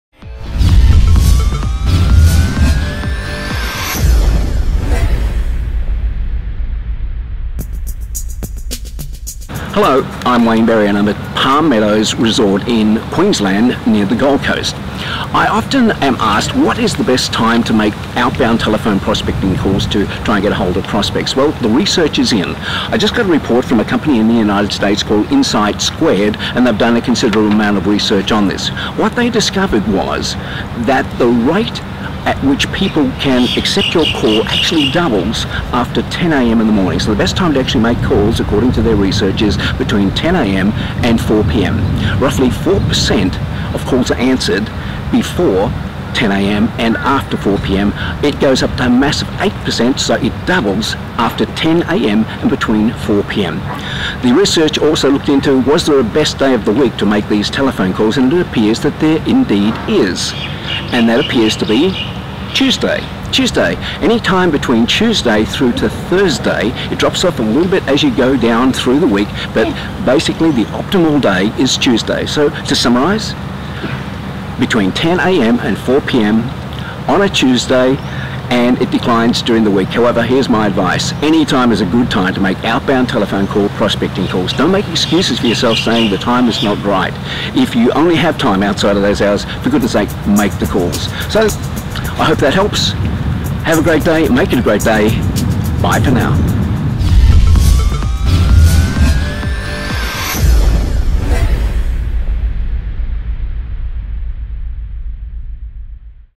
Best time to make prospecting calls by telephone
Wayne Berry explains that the research is in.  There is a best time and day to make telephone prospecting calls as this USA Research Study shows.